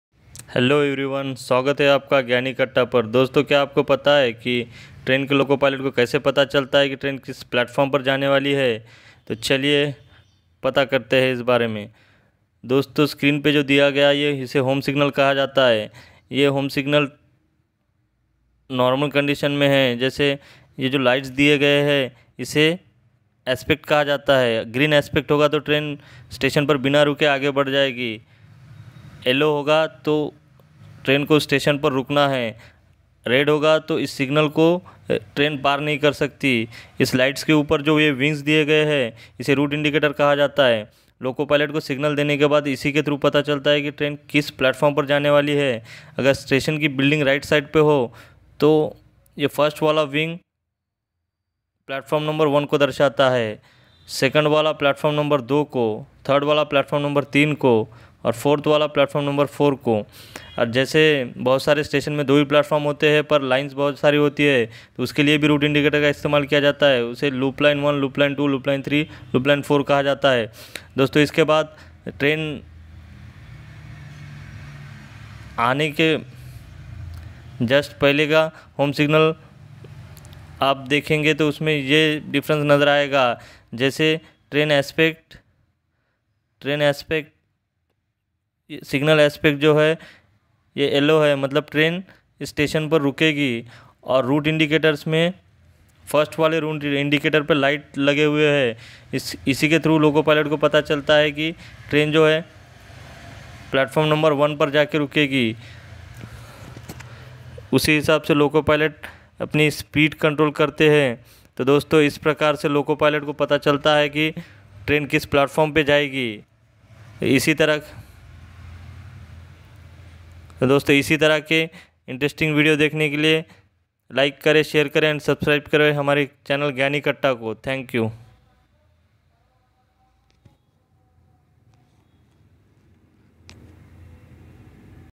loco pilot ko kaise pata chalta hai ki station aane wala hai l train kis platform par jane wali hai
Hello everyone,
welcome to Gyani Katta in this video you will understand that how train Loco pilot knows that station is coming and train going to wich platform
just seeing Home signal.